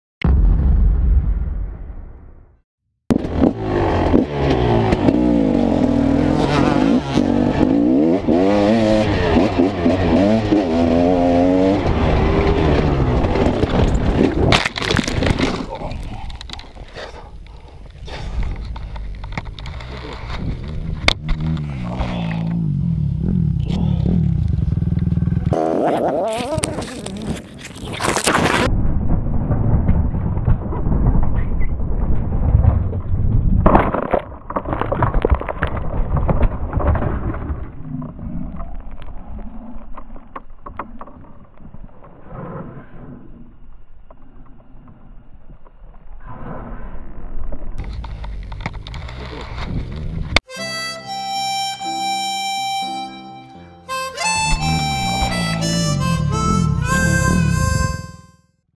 KTM EXC 300 crash recorded with GoPro Hero3
The first "serious" fall with my KTM 300 EXC 2012 and GoPro Hero3 Black Edition on the helmet. It was my fault because I did not examine the terrain at first. Helmet visor broke and stuck under my nose where I got ten stitches.